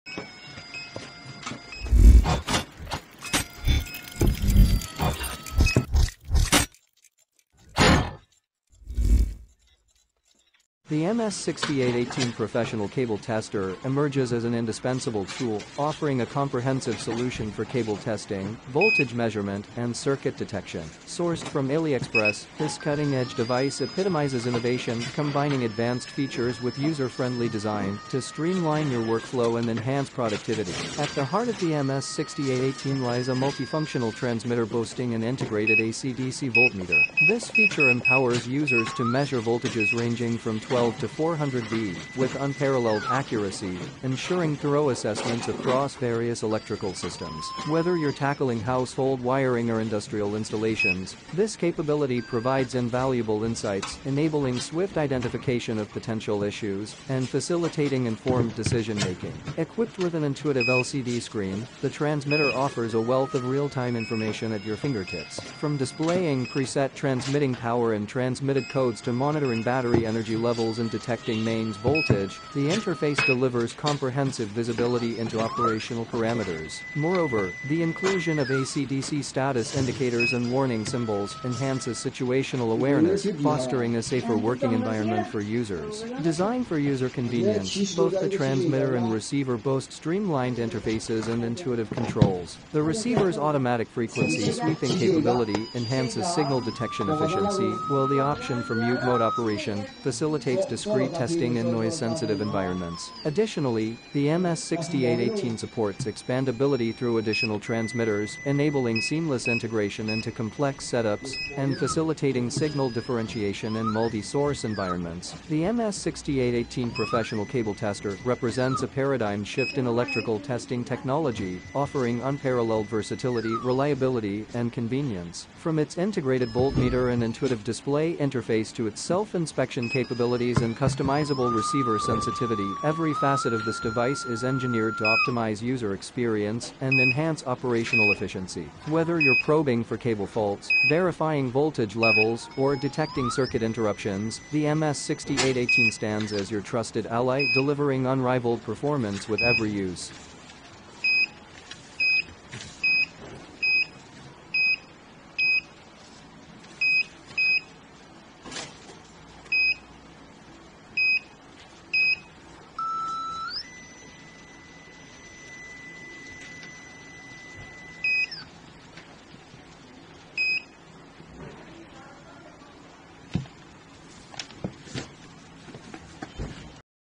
Test MS6818 Professional Cable Tester Review Aliexpress
Test MS6818 Professional Cable Tester 12-400V AC/DC Metal Pipe Locator Wall Underground Inspect Interruption Short Circuit Detection Unboxing Aliexpress, The MS6818 Professional Cable Tester available on AliExpress is a comprehensive tool designed for electrical and construction tasks. The transmitter boasts a built-in AC/DC voltmeter capable of measuring voltage within the range of 12 to 400 volts. Its LCD screen displays various parameters such as transmitting power, codes, battery level, mains voltage, and working status. Additionally, the transmitter conducts self-inspection and displays the results for user reference. The receiver offers adjustable sensitivity and automatic frequency sweeping, operating under mute mode for convenience. Furthermore, the system allows for expansion with additional transmitters for signal differentiation. The specifications include voltage measurement range, cable depth measurement, and environmental conditions for optimal performance. Powered by batteries, the MS6818 is durable, compact, and portable, making it suitable for a range of professional applications.
MS6818 Professional Cable Tester Package List:
1 x MS6818 Receiver
1 x Transmitter
1 x Test Leads(Black & Red)
1 x Test Probe(Black & Red)
1 x Crocodile Clamps(Black & Red)
1 x Grounding Rod
1 x User Manual